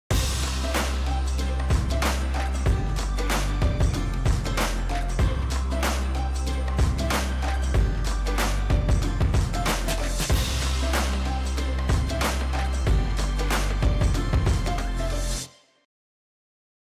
PackHack for rugs | PODS
Pack and move your area rugs the right way with our 5-step guide on how to clean, protect, and move your rugs to your new home.